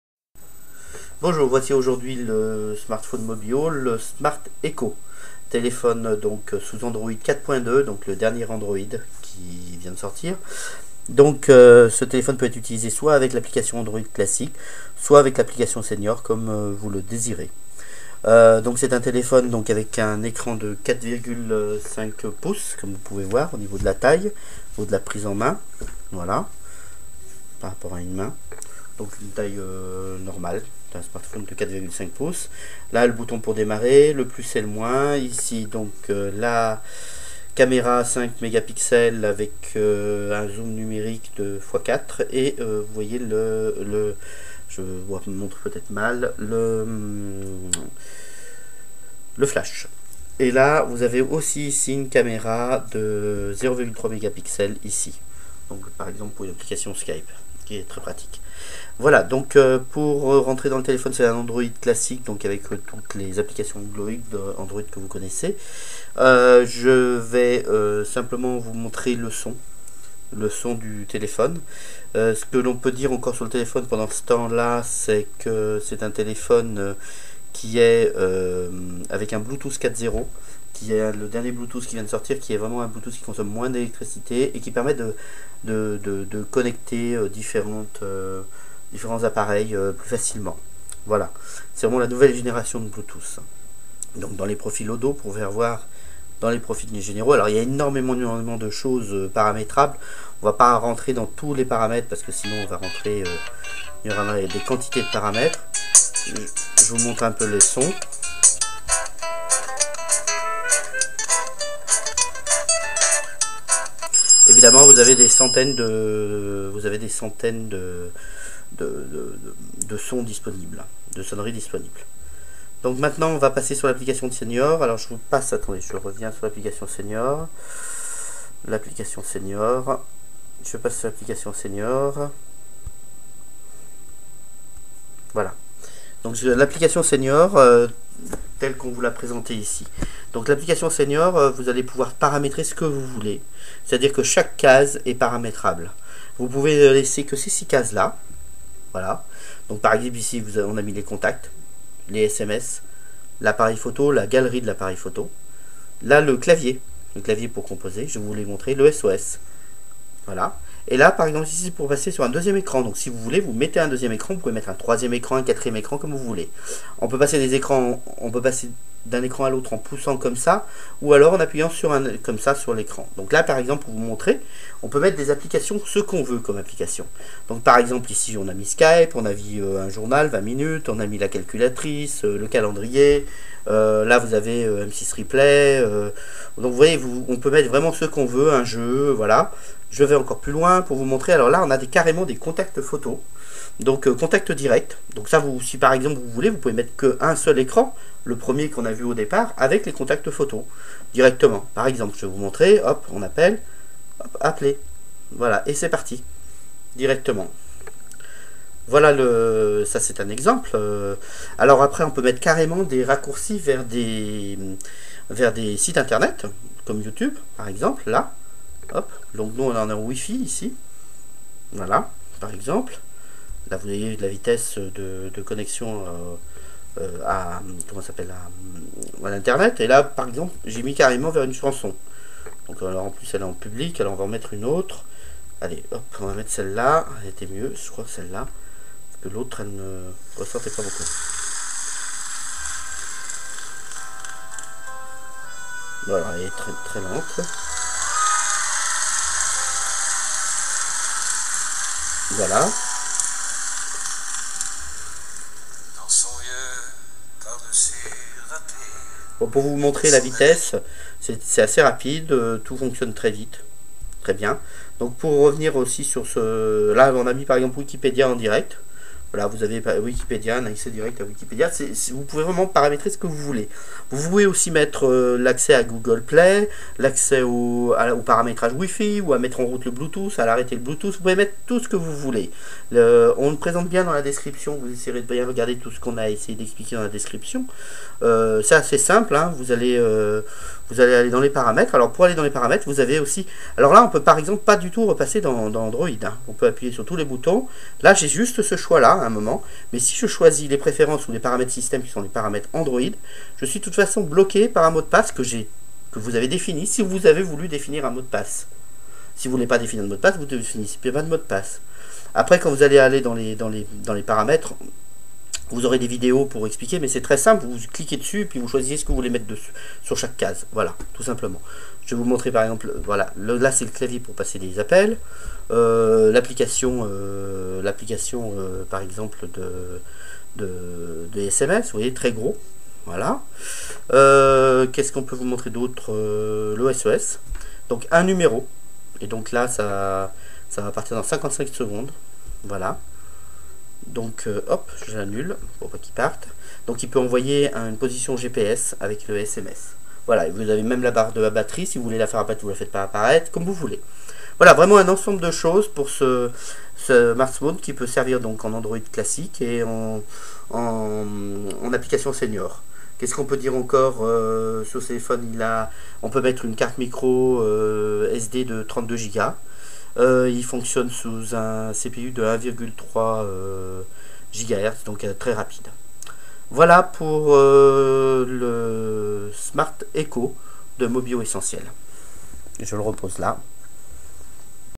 Smartphone pour senior, SmartEcho
PASOLO.com vous propose de découvrir le smartphone pour senior, SmartEcho Il s'agit d'un téléphone pour senior avec Androïd 4.2.